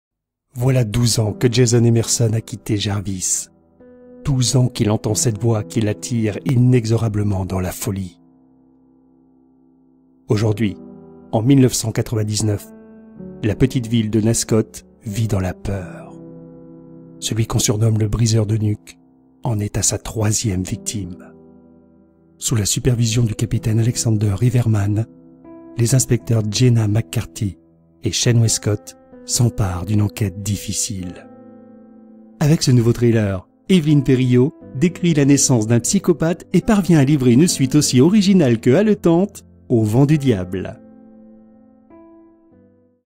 L'oeil du diable
Voilà douze ans que Jason Emerson a quitté Jarvis, douze ans qu'il entend cette voix qui l'attire inexorablement dans la folie...

Aujourd'hui, en 1999, la petite ville de Nascoth vit dans la peur : celui qu'on surnomme « le Briseur de nuque » en est à sa troisième victime.

Sous la supervision du capitaine Alexander Riverman, les inspecteurs Jenna McCarty et Shane Wescott s'emparent d'une enquête difficile...

Avec ce nouveau thriller, Évelyne Peyrillaud décrit la naissance d'un psychopathe et parvient à livrer une suite aussi originale que haletante aux Vents du diable.